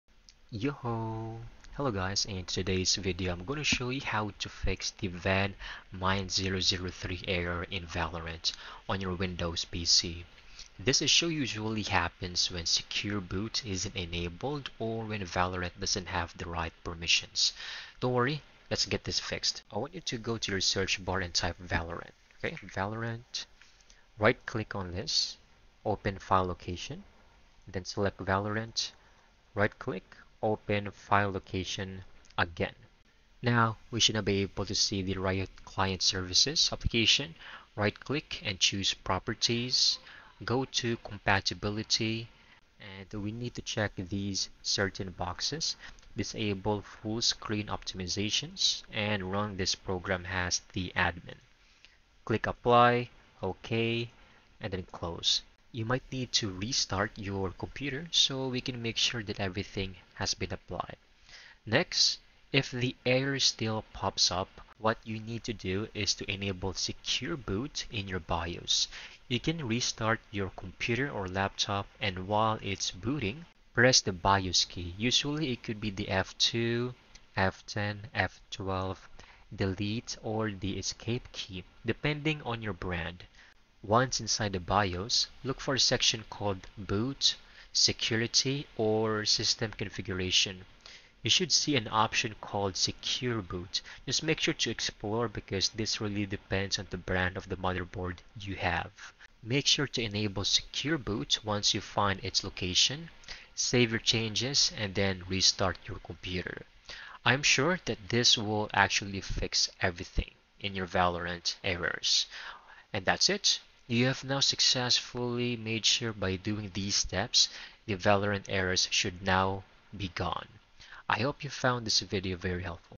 How to Solve Valorant VAN9003 Secure Boot / UEFI Error on Windows
Fix Valorant Error VAN9003 on Windows — Secure Boot / UEFI Solution in 2026

If you’re seeing the VAN9003 Secure Boot / UEFI Error when trying to launch Valorant, it means that Riot Vanguard, the game’s anti-cheat system, has detected that Secure Boot or TPM 2.0 isn’t enabled on your system. These security features are mandatory for Valorant on Windows 11 (and sometimes Windows 10) to prevent cheats or system tampering.

This complete 2025 guide explains exactly how to fix the Valorant VAN9003 error by enabling Secure Boot and TPM, updating BIOS settings, and getting back into the game safely.